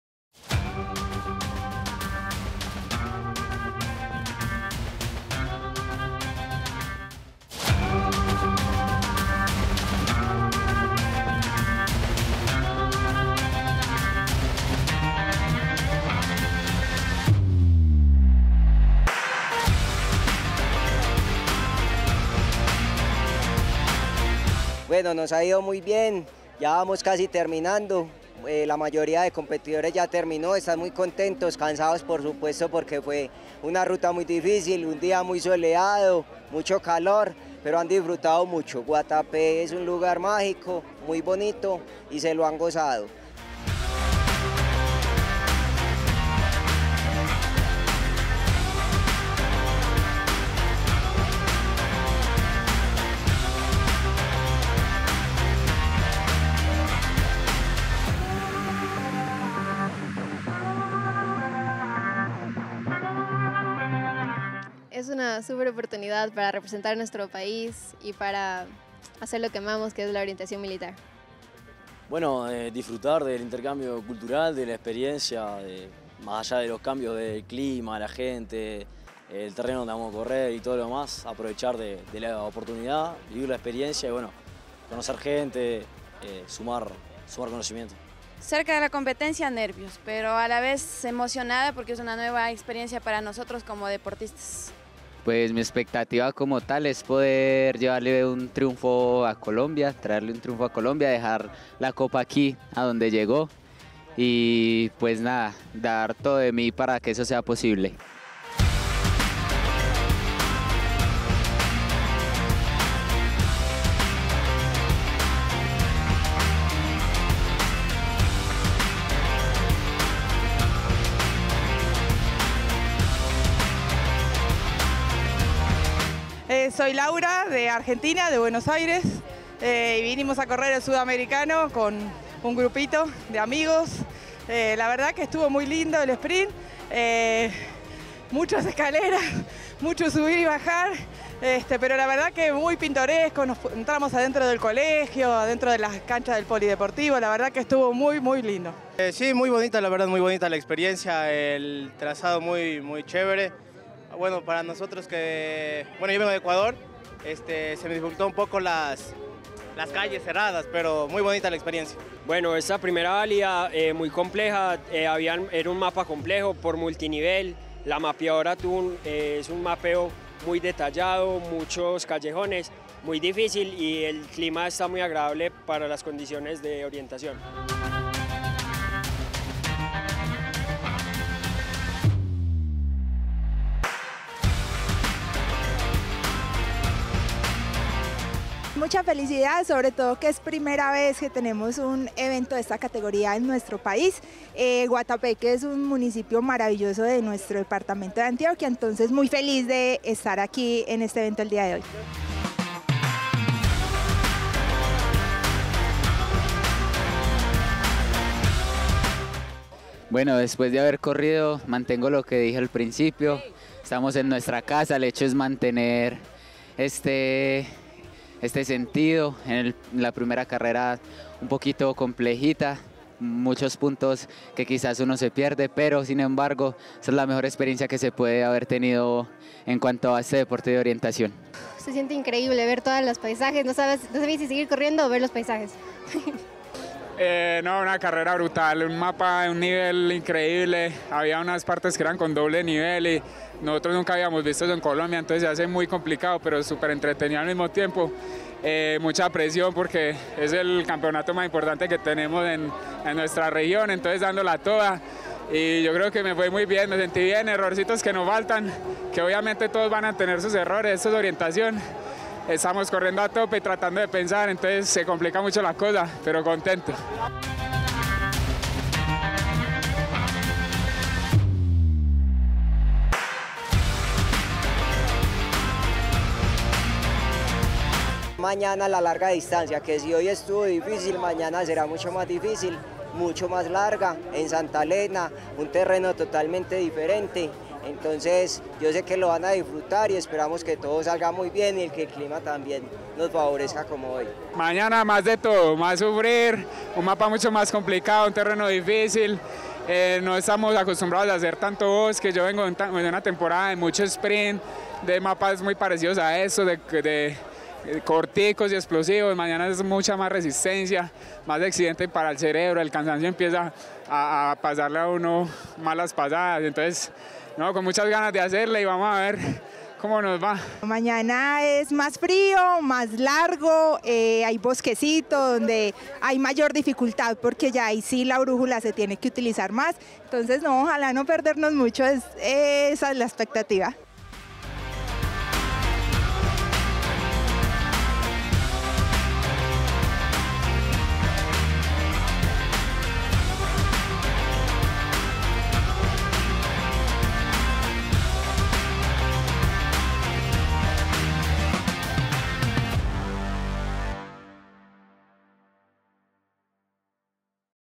V Campeonato Suramericano de Orientación | Día 1 | CCM Televisión
V Campeonato Suramericano de Orientación | Día 1

Entre el 11 y el 14 de octubre, Marinilla y el Oriente Antioqueño recibirán a
cerca de 300 deportistas de 24 países que, desde los cinco continentes, han
llegado hasta aquí para competir en la mayor cita de orientación de nuestro
continente, un evento que, además, será válido para el ranking mundial de la
Federación Internacional de Orientación (IOF).

Conéctate a través de CCMTelevisión ¡Para ver lo que somos!